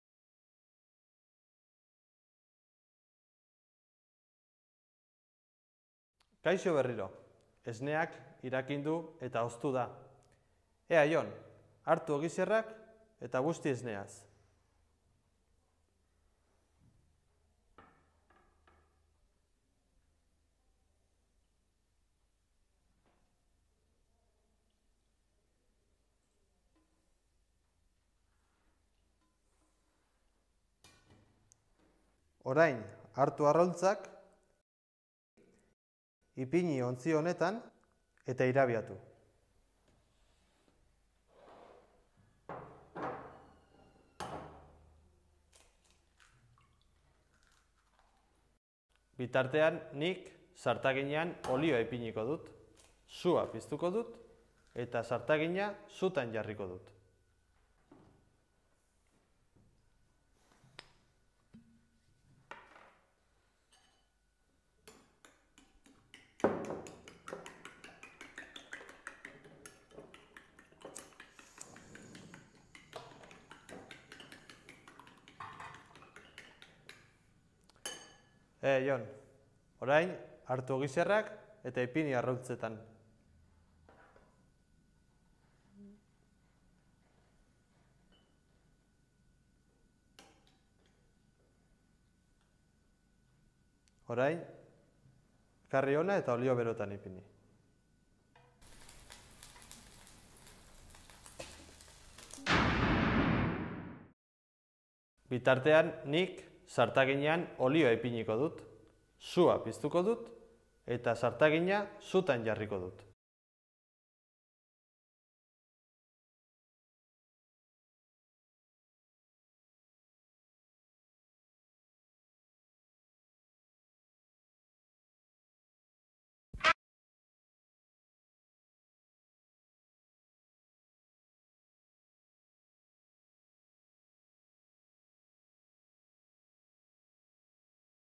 MTB-3 Inauteriak atzo eta gaur (3. atala)
Euskaraz abiatzeko oinarriak.

Ikusentzunezko honek eta berarekin egindako ikasmaterialak Hezkuntza, Unibertsitate eta Ikerketa-Sailaren egokitasun-aitormena dauka.

3.atala: denborazko aldiberekoak, aditza etorkizunean.

Irakasmaila: DBH
Jakintzagaia: Euskal Hizkuntza.